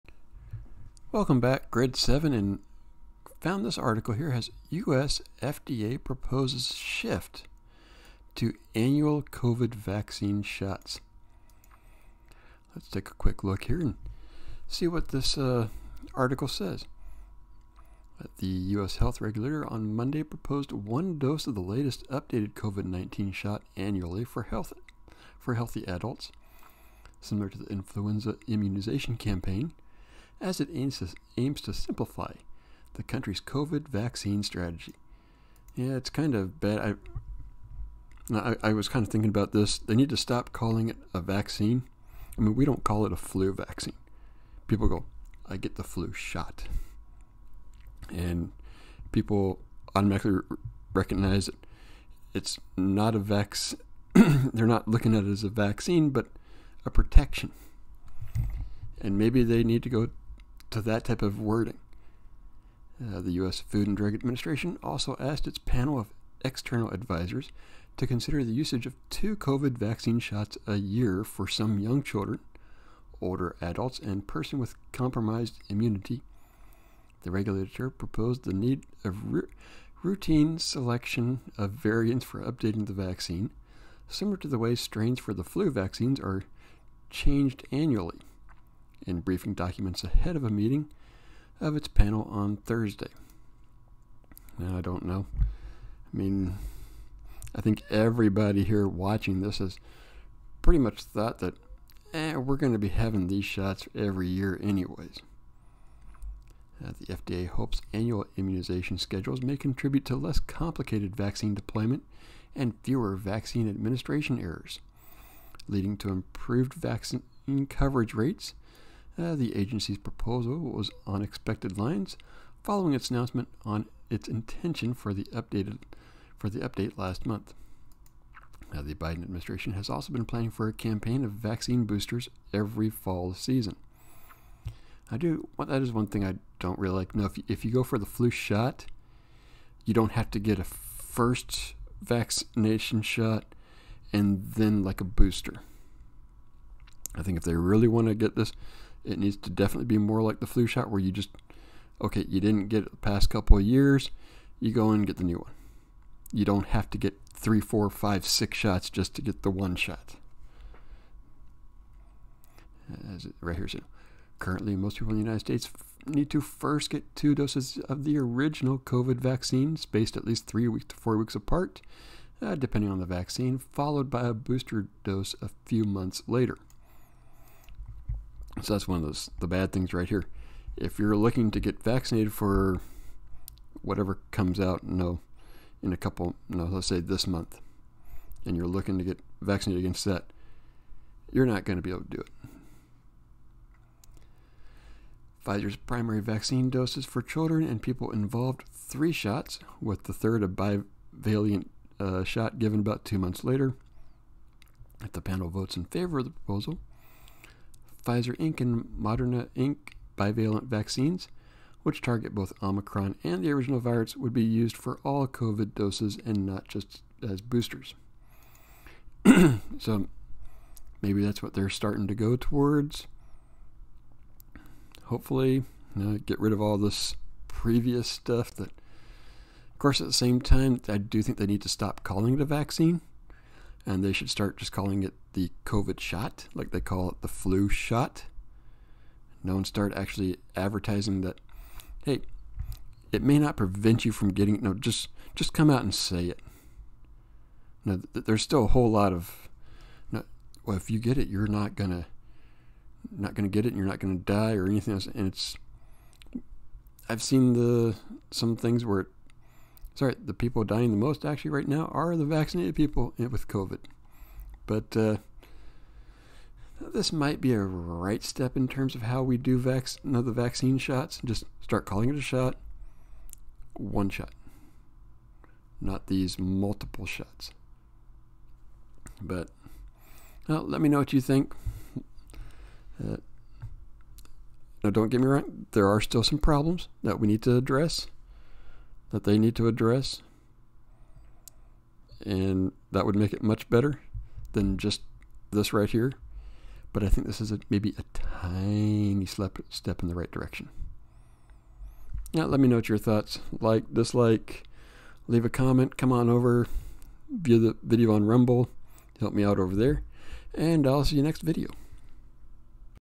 FDA Proposing Annual Covid Vaccine Shots
FDA is now proposing what many already knew would happen, an annual vaccine shot.